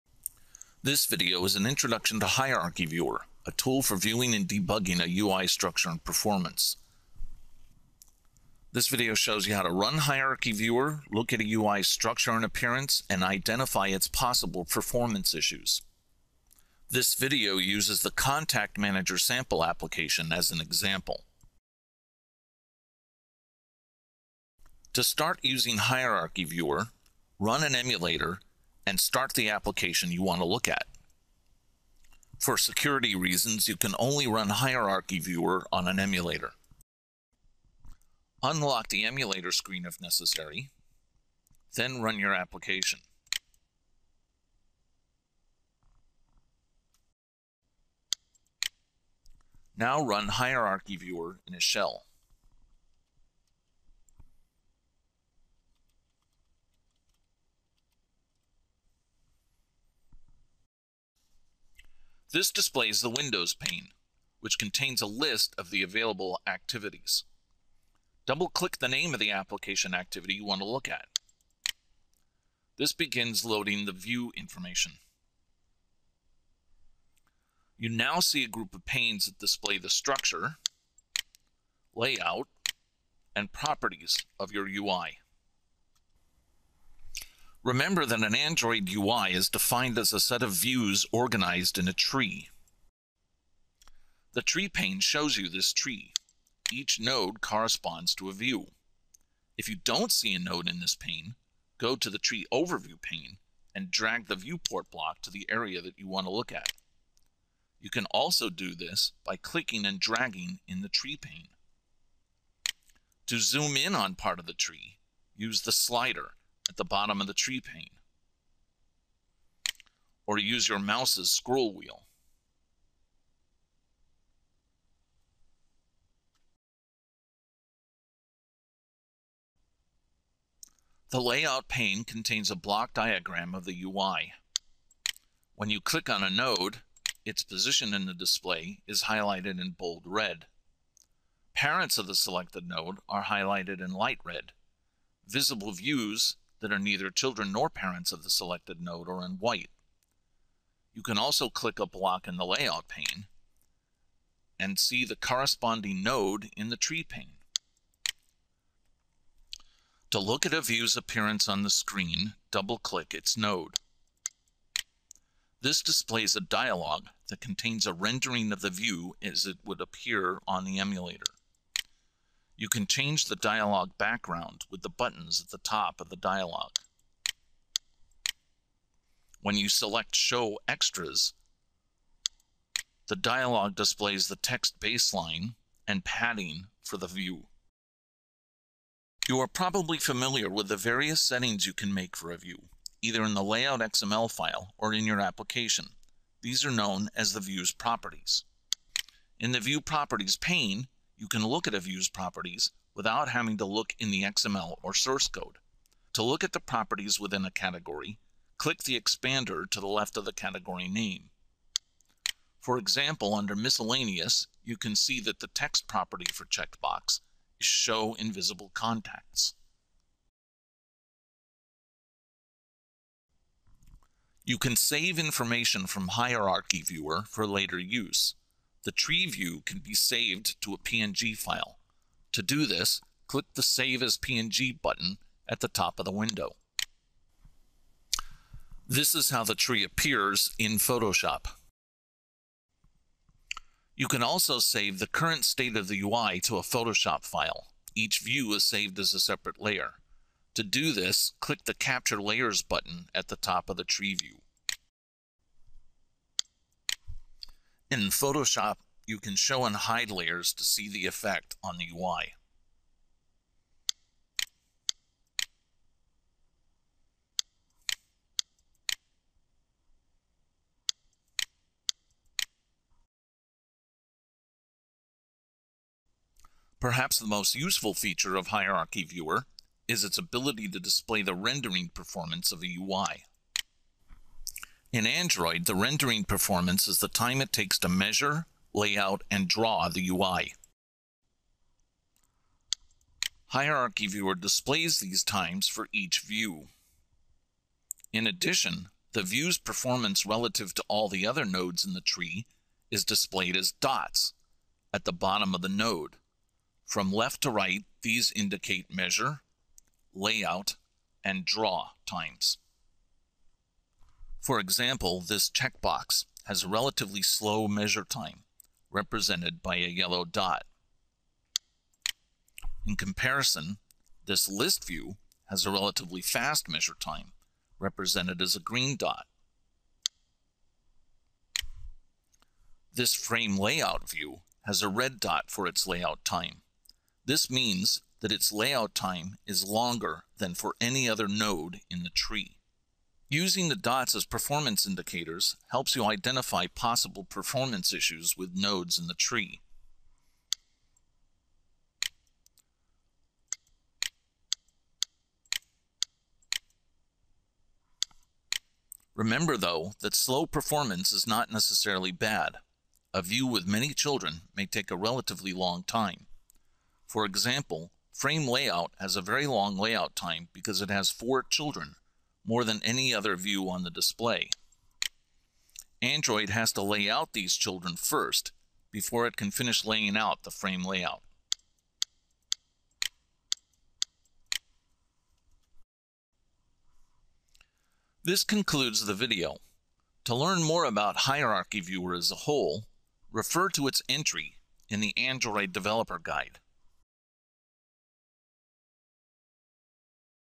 Hierarchyviewer
This video is an introduction to hierarchyviewer, an Android SDK tool that displays an application's View properties, the hierarchy of views, and the display performance of views.